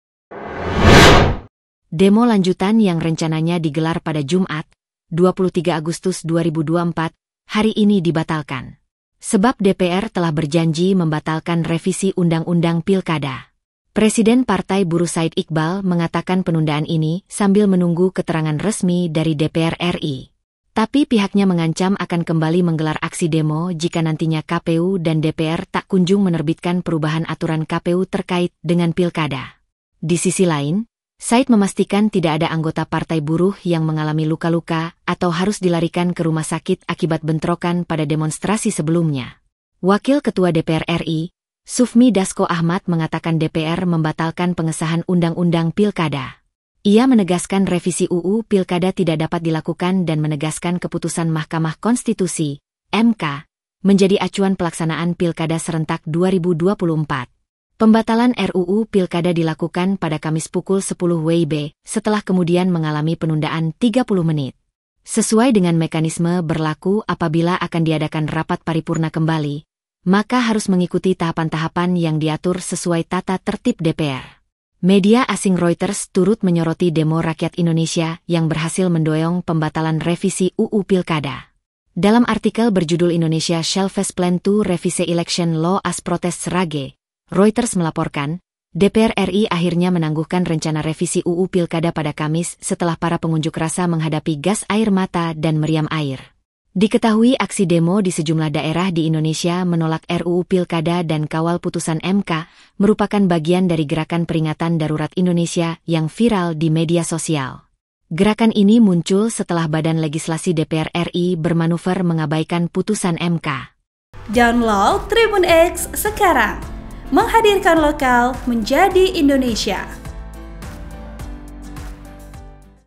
Aksi Unjuk Rasa di Depan KPU dan DPR RI Hari Ini Ditunda
TRIBUN-VIDEO.COM - Demo lanjutan yang rencananya digelar pada Jumat (23/8/2024) hari ini dibatalkan.

Sebab DPR telah berjanji membatalkan revisi Undang-Undang Pilkada.

Presiden Partai Buruh Said Iqbal mengatakan penundaan ini sambil menunggu keterangan resmi dari DPR RI.

Tapi pihaknya mengancam akan kembali menggelar aksi demo jika nantinya KPU dan DPR tak kunjung menerbitkan perubahan aturan KPU terkait dengan Pilkada.

Di sisi lain, Said memastikan tidak ada anggota Partai Buruh yang mengalami luka-luka atau harus dilarikan ke rumah sakit akibat bentrokan pada demonstrasi sebelumnya.

Wakil Ketua DPR RI, Sufmi Dasco Ahmad mengatakan DPR membatalkan pengesahan Undang-undang Pilkada.

Ia menegaskan revisi UU Pilkada tidak dapat dilakukan dan menegaskan keputusan Mahkamah Konstitusi (MK) menjadi acuan pelaksanaan Pilkada Serentak 2024.

Pembatalan RUU Pilkada dilakukan pada Kamis pukul 10 WIB setelah kemudian mengalami penundaan 30 menit.

Sesuai dengan mekanisme berlaku apabila akan diadakan rapat paripurna kembali, maka harus mengikuti tahapan-tahapan yang diatur sesuai tata tertib DPR.

Media asing Reuters turut menyoroti demo rakyat Indonesia yang berhasil mendoeong pembatalan Revisi UU Pilkada.

Dalam artikel berjudul Indonesia shelves plan to revise election law as protests rage, Reuters melaporkan, DPR RI akhirnya menangguhkan rencana revisi UU Pilkada pada Kamis setelah para pengunjuk rasa menghadapi gas air mata dan meriam air.

Gerakan ini muncul setelah Badan Legislasi DPR RI bermanuver mengabaikan putusan MK.

Sumber Berita:
Naskah Video :